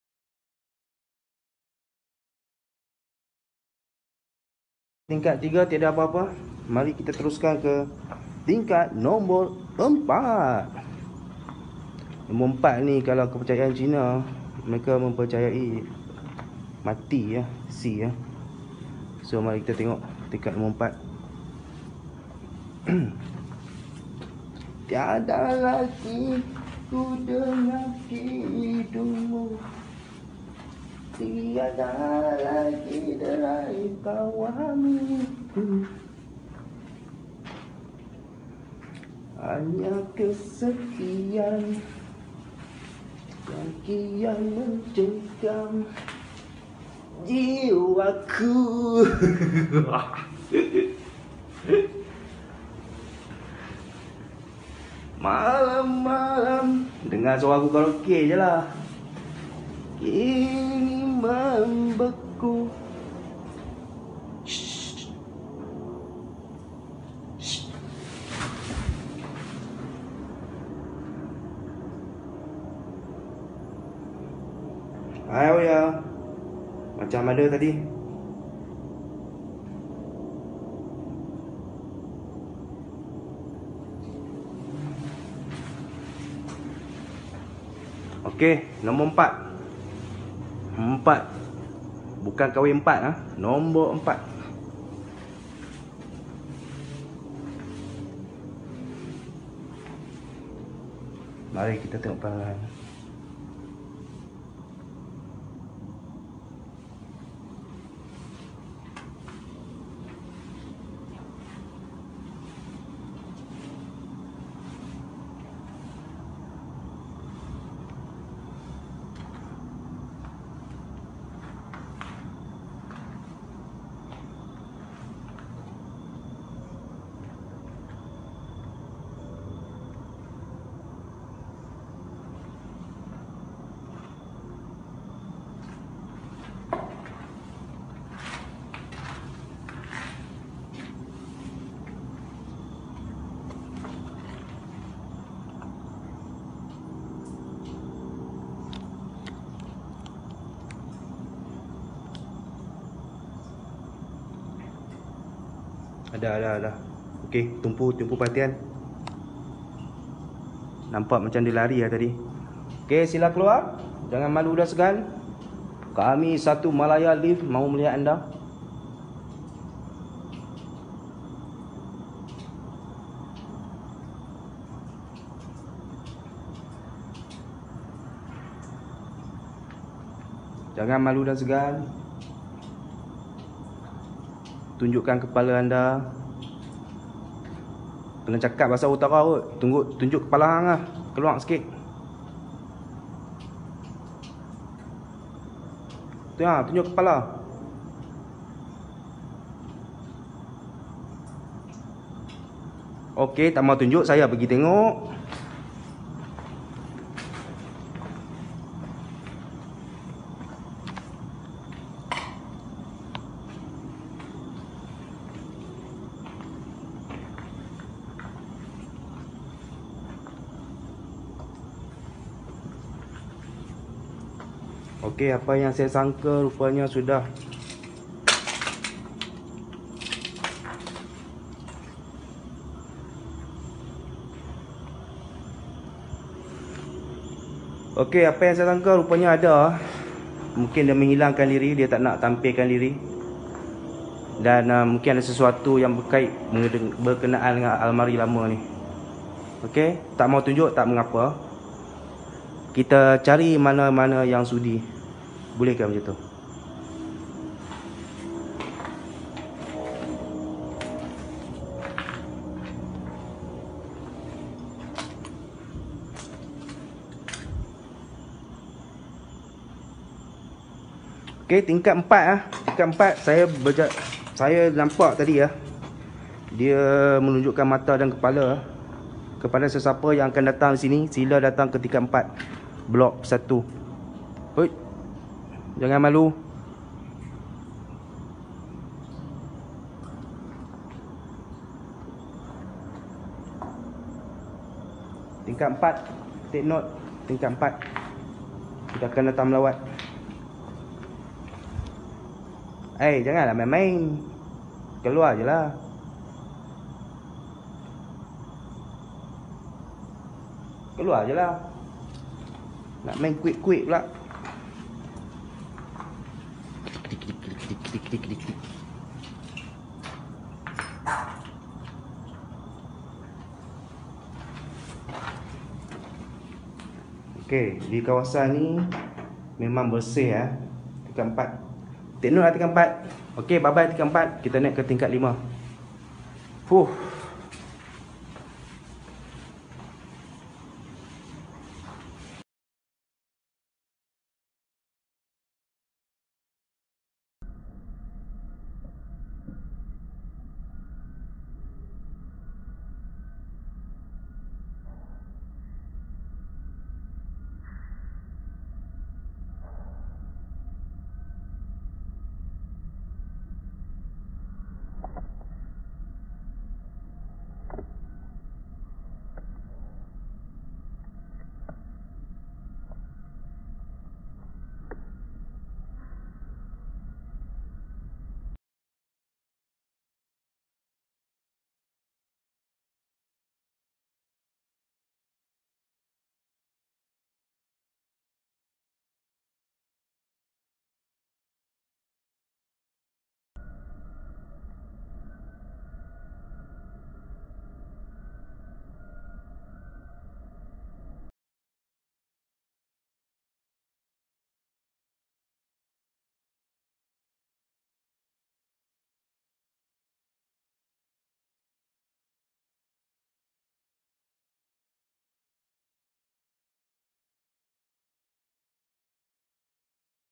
Solo Ambush at Higland Tower, Lembaga Berkepala Hitam!!!
Lokasi di Highland Tower, di tingkat 4 lembaga hitam yg bergerak pantas dan bersembunyi.